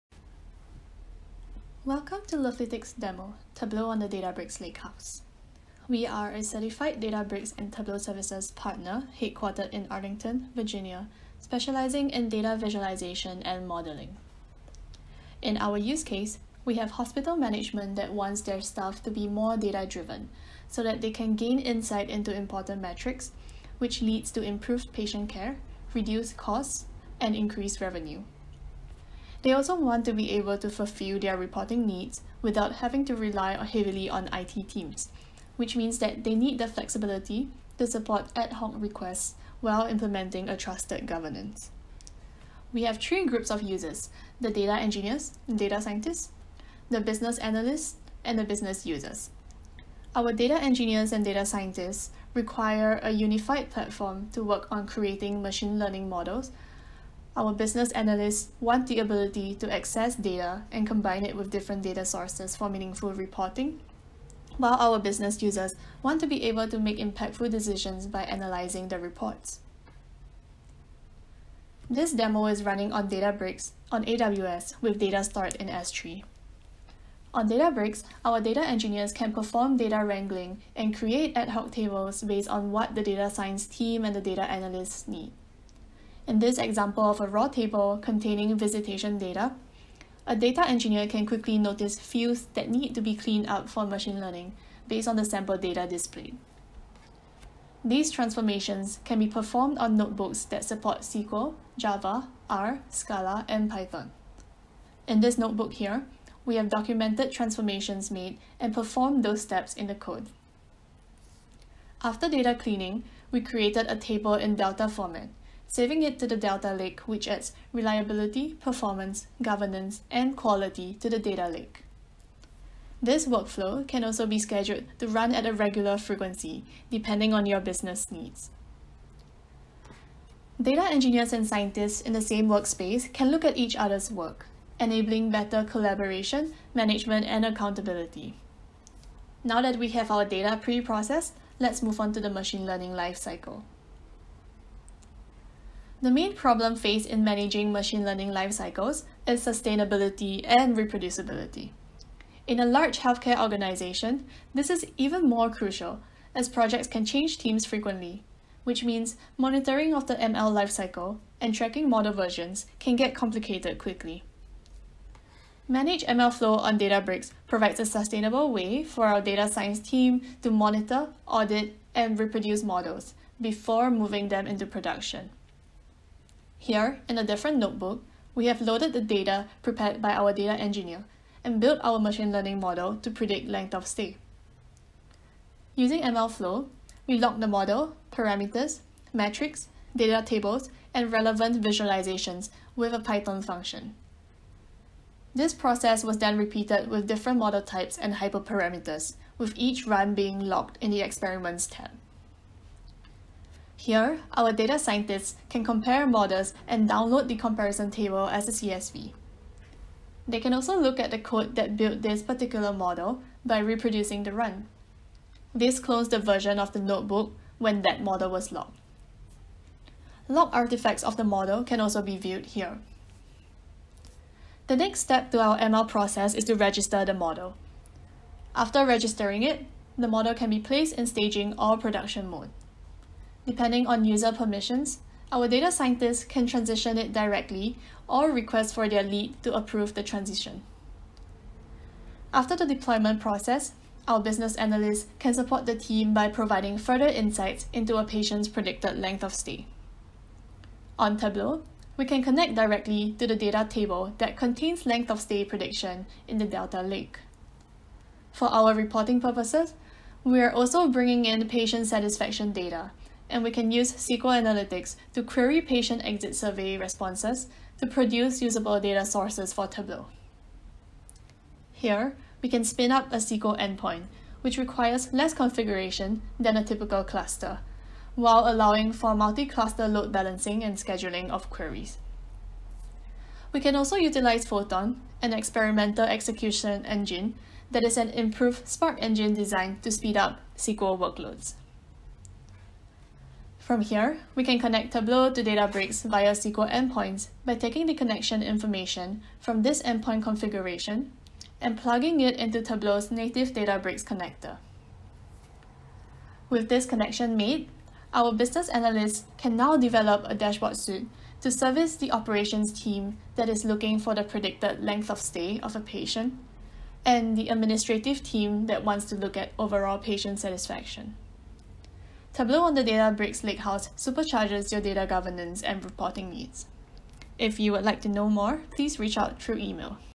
Tableau on the Databricks Lakehouse : Presented by Lovelytics
Have you ever wanted to simplify your data science and analytics process? Do you need to find a better way to enable both your technical and non-technical users?

See how the @lovelytics8302 team uses @Databricks and @Tableau to enable organizations with analytics on the Lakehouse.

In this 5 minute demo, learn how organizations are utilizing Databricks and Tableau to store, transform, model, and integrate data for machine learning and visualization.